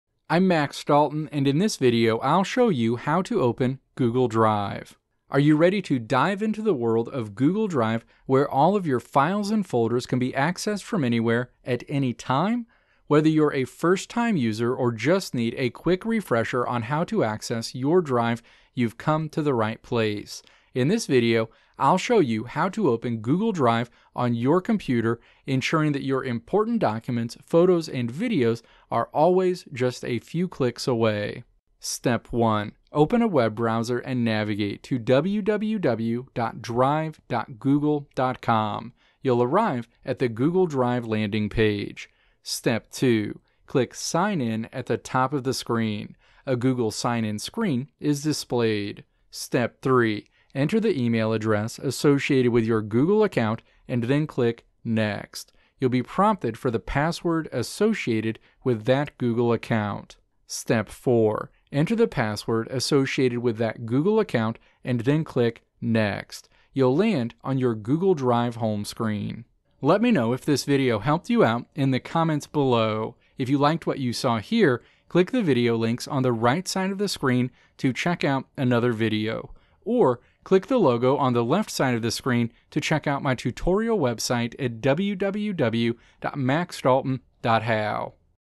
How to Open Google Drive on Your Computer
In this video I'll show you how to open Google Drive on your computer.

Timestamps:
Introduction: 0:00
Steps to Open Google Drive on Your Computer: 0:31
Conclusion: 1:09

Video Transcript:
Are you ready to dive into the world of Google Drive, where all your files and folders can be accessed from anywhere, at any time? Whether you're a first-time user or just need a quick refresher on how to access your drive, you've come to the right place.

In this video I'll show you how to open Google Drive on both your computer, ensuring that your important documents, photos, and videos are always just a few clicks away.

Step 1. You'll arrive at the Google Drive landing page.

Step 2. Click "Sign In" at the top of the screen. A Google sign-in screen is displayed.

Step 3. You'll be prompted for the password associated with that Google account.

Step 4. Enter the password associated with that Google account, and then click "Next." You'll land on your Google Drive home screen.